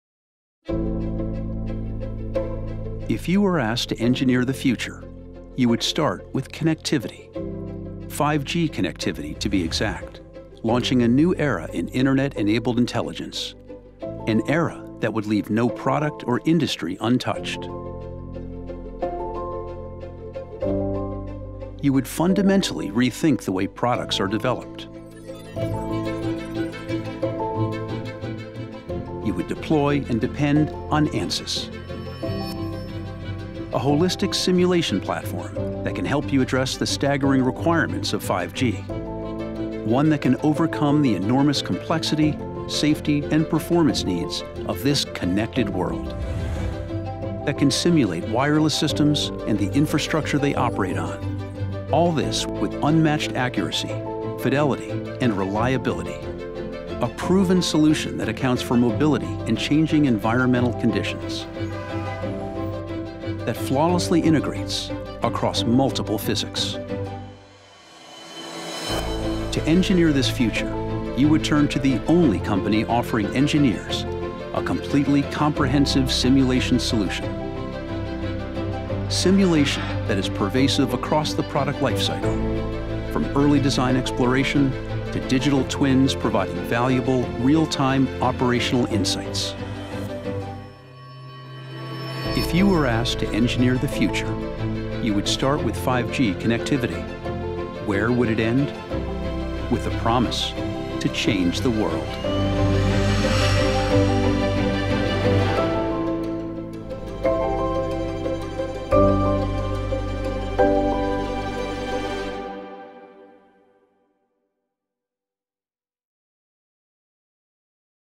5G Connectivity with ANSYS
Delivering on the promise of 5G will require engineers to overcome significant design challenges for user equipment, network and data center development. Learn how ANSYS pervasive simulation solutions can help your 5G development efforts.